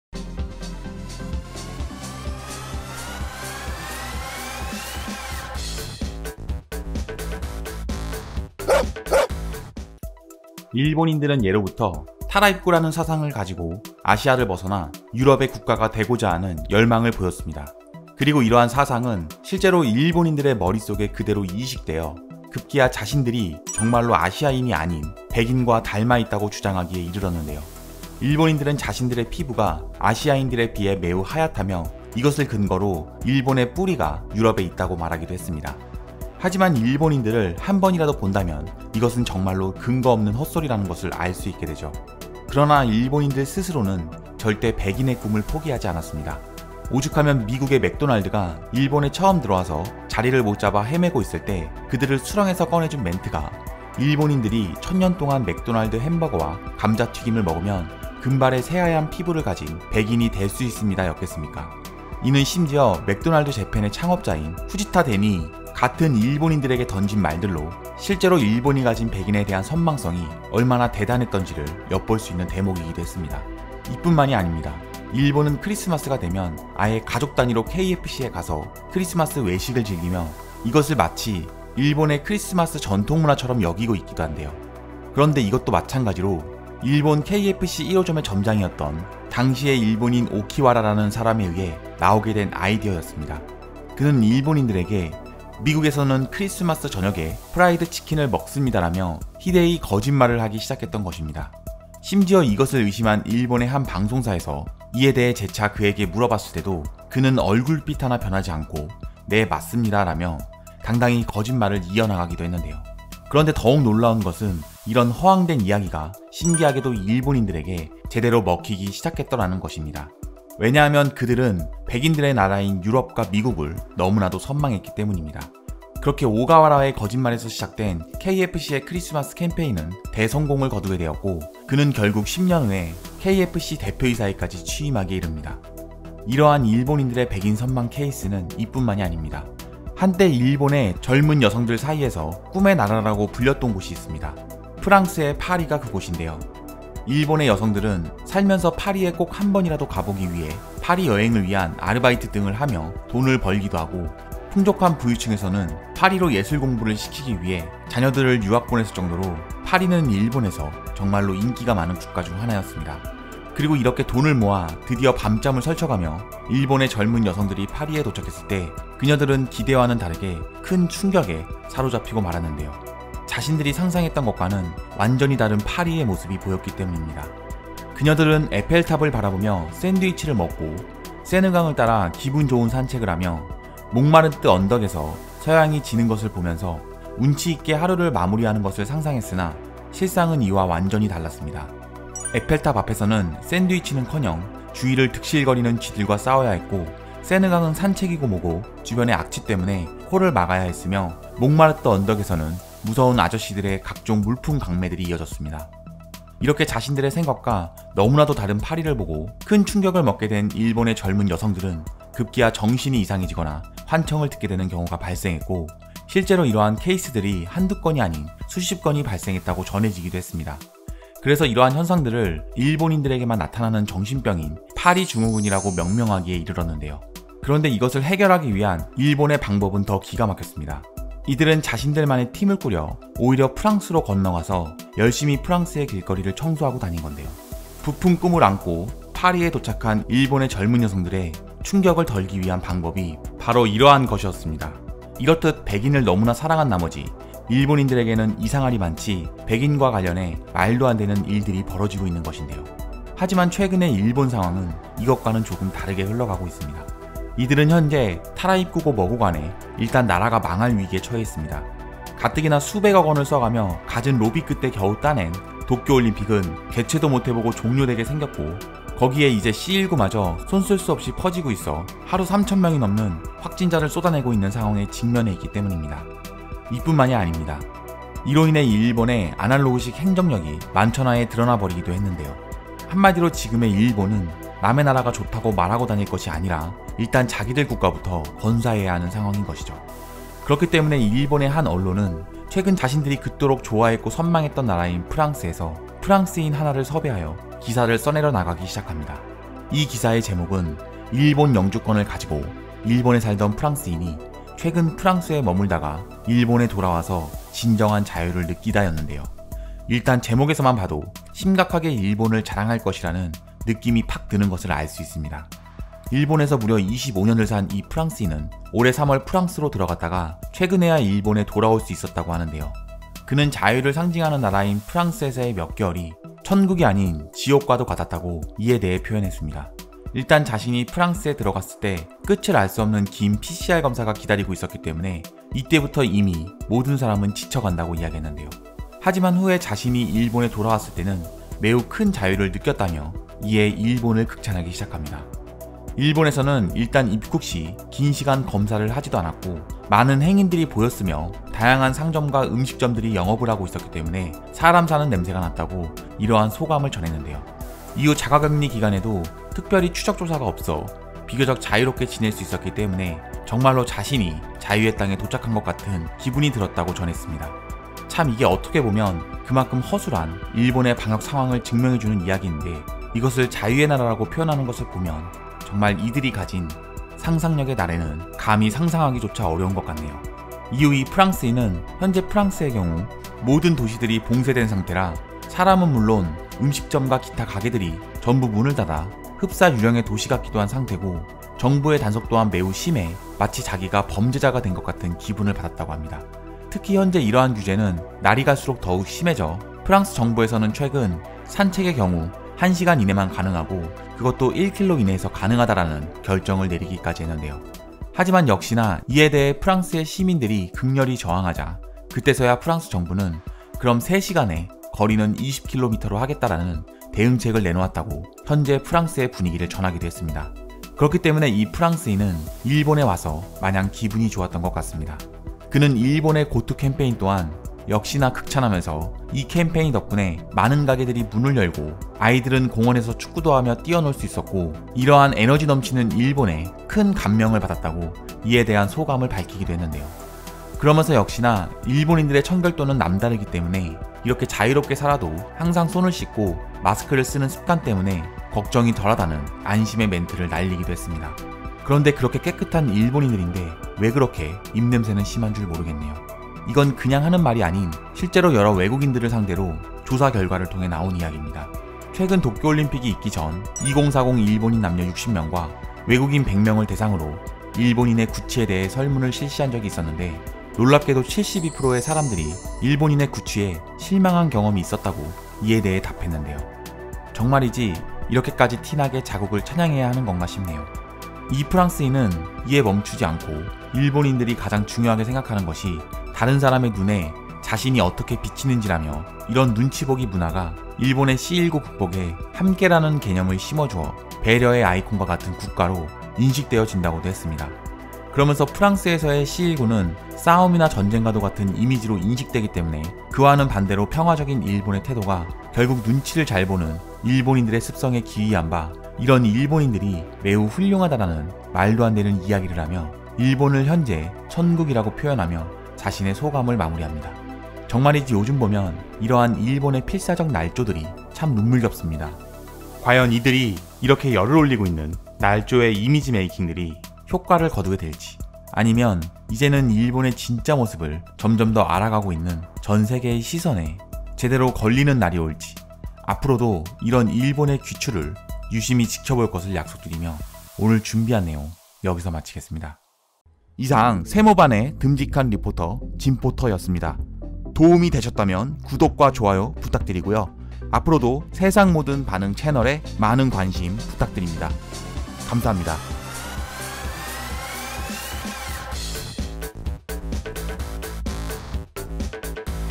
일본에서 25년을 살던 프랑스인이 최근 일본의 진짜 모습을 목격하고 눈물 흘린 이유
안녕하세요.
대한민국에 관한 세상 모든 반응을
모아 전달하는 진포터입니다.